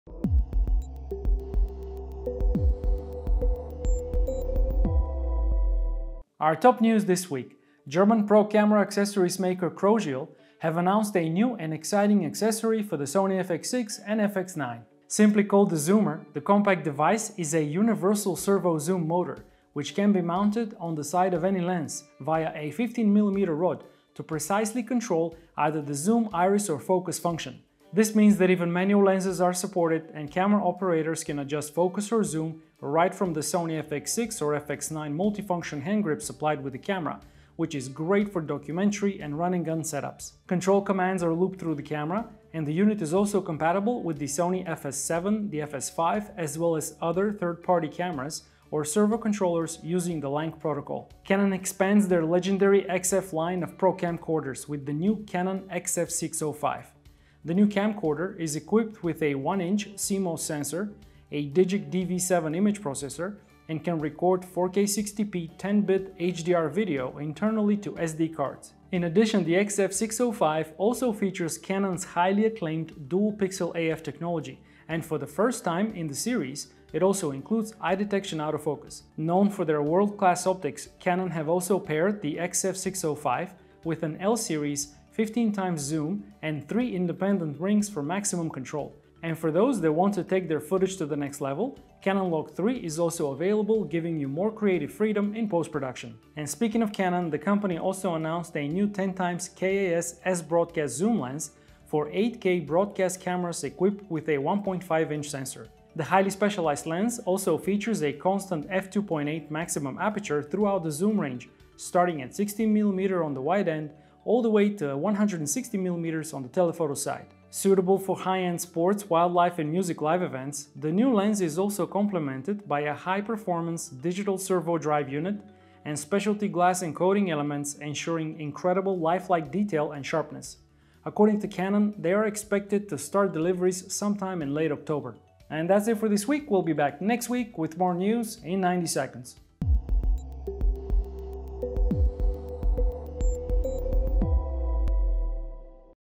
News in 90 EP 247: Chrosziel Zoomer, Canon XF605, Canon 10x16 KAS S
Chrosziel Zoomer: 0:00
Canon XF605: 0:49
Canon 10x16 KAS S: 1:35